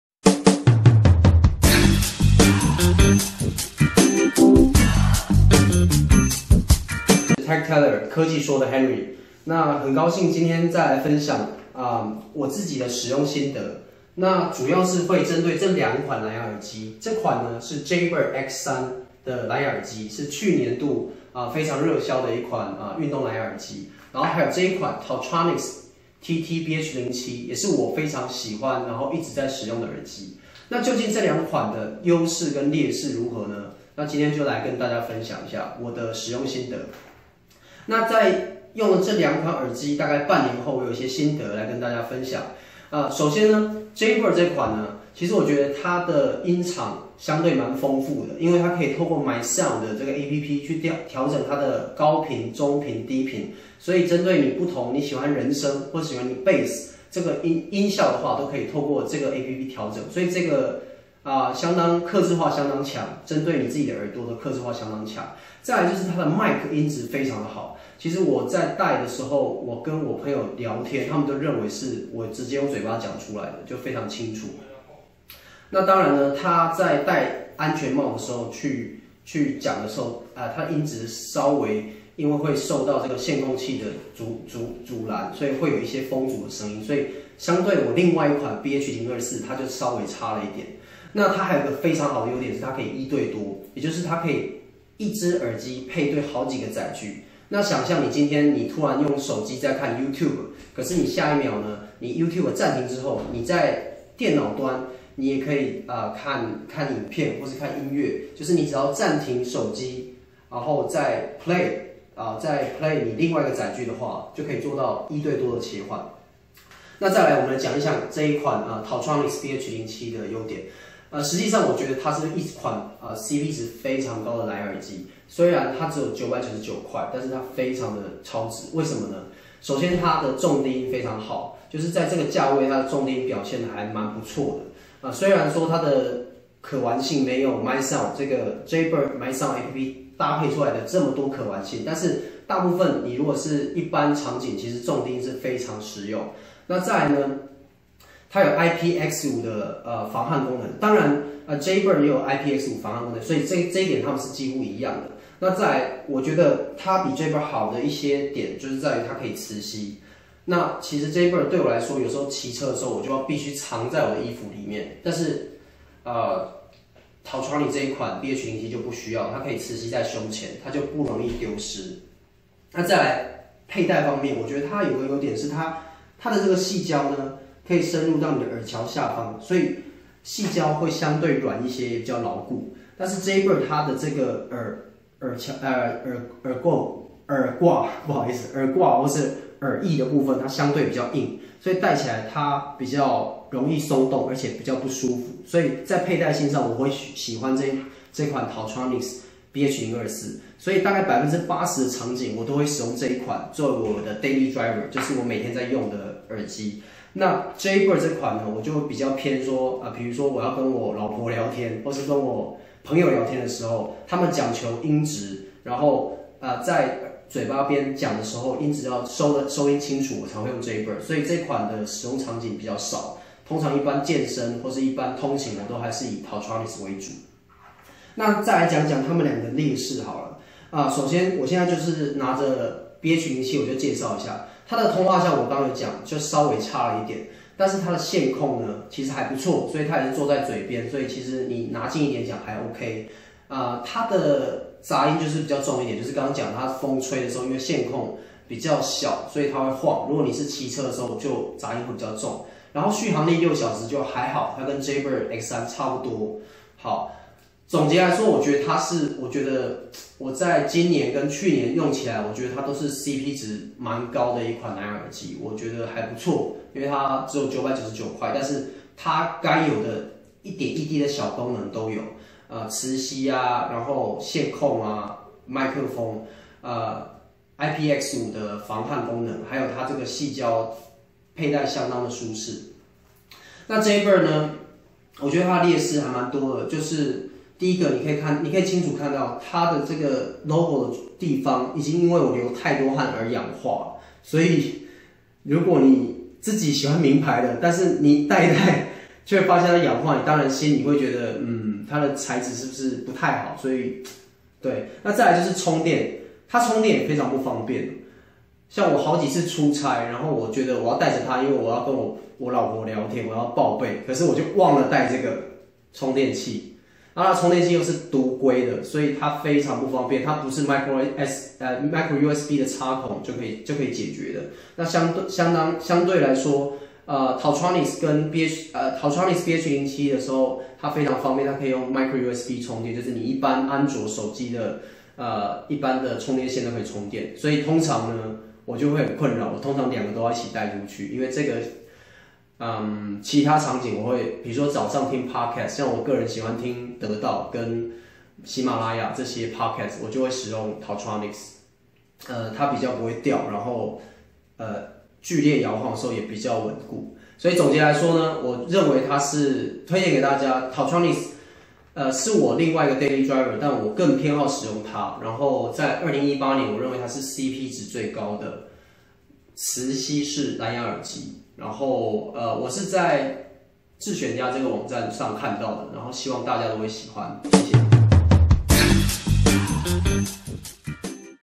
2019年CP值最高的藍牙耳機推薦 - TaoTronics TT-BH07 vs JayBird X3 大車拼
‼️ 影片未經授權，禁止轉載 ‼️

2019年CP值最高的藍牙耳機  - 頒給Taotronics TT-BH07
亨利給87分不能再高了

👑 矽膠耳勾佩戴好舒服
👑 磁吸佩戴防丟失
👑 重低音不含糊
👑 IPX5防汗功能
👑 線控MIC收音水準之上